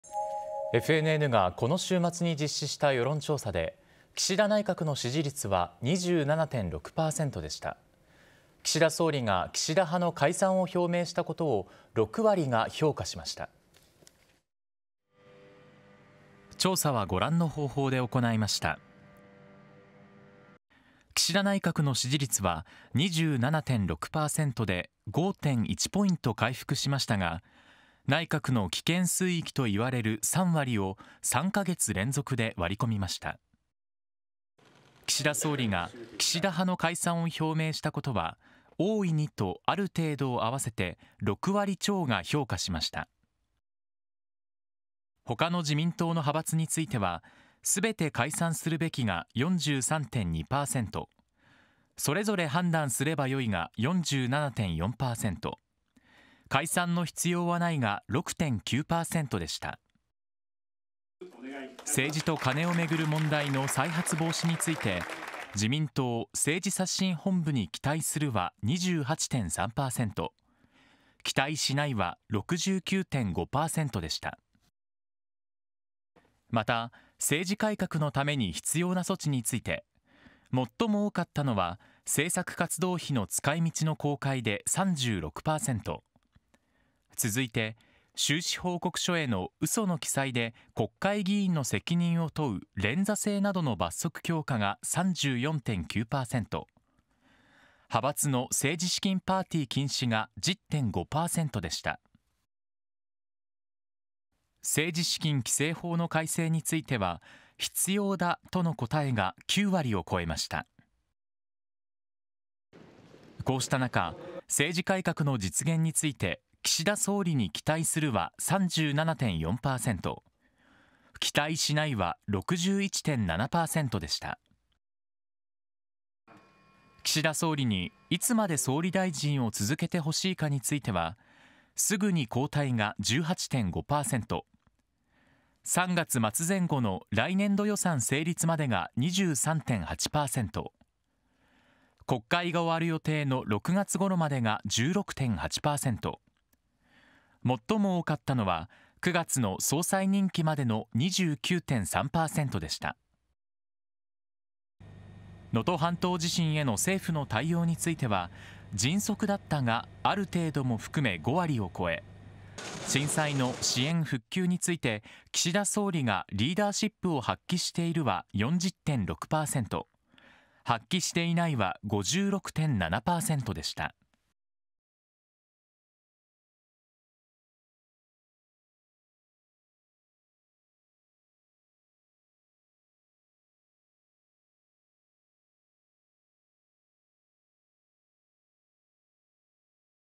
6割が「岸田派解散」を評価 内閣支持率27.6％に 5.1ポイント回復も…「危険水域」を3カ月連続割り込み FNN世論調査
FNNがこの週末に実施した世論調査で、岸田内閣の支持率は27.6％だった。

岸田首相が、岸田派の解散を表明したことを6割が「評価」した。

調査は、以下の方法で行った。
・期間: 2024年1月20日・21日
・電話調査(RDD 固定・携帯電話)
・全国18歳以上の男女1,019人が回答

岸田内閣の支持率は27.6％で、5.1ポイント回復したが、内閣の「危険水域」といわれる3割を3カ月連続で割り込んだ。

岸田首相が岸田派の解散を表明したことは、「大いに」と「ある程度」をあわせて6割超が「評価」した。

ほかの自民党の派閥については、「すべて解散するべき」が43.2％、「それぞれ判断すれば良い」が47.4％、「解散の必要はない」が6.9%だった。

政治と金をめぐる問題の再発防止について、自民党「政治刷新本部」に「期待する」は28.3％、「期待しない」は69.5％だった。

また、政治改革のために最も必要な措置について、最も多かったのは「政策活動費の使い道の公開」で36％、続いて「収支報告書へのうその記載で国会議員の責任を問う連座制などの罰則強化」が34.9％、「派閥の政治資金パーティー禁止」が10.5％だった。

政治資金規正法の改正については、「必要だ」との答えが9割を超えた。

こうした中、政治改革の実現について、岸田首相に「期待する」は37.4％、「期待しない」は61.7％だった。

岸田首相にいつまで首相を続けてほしいかについては、「すぐに交代」が18.5％、「3月末前後の2024年度予算成立まで」が23.8％、「国会が終わる予定の6月ごろまで」が16.8%で、最も多かったのは「9月の総裁任期まで」の29.3％だった。

能登半島地震への政府の対応については、「迅速だった」が「ある程度」も含め5割を超え、震災の支援・復旧について、岸田首相がリーダーシップを「発揮している」は40.6％、「発揮していない」は56.7％だった。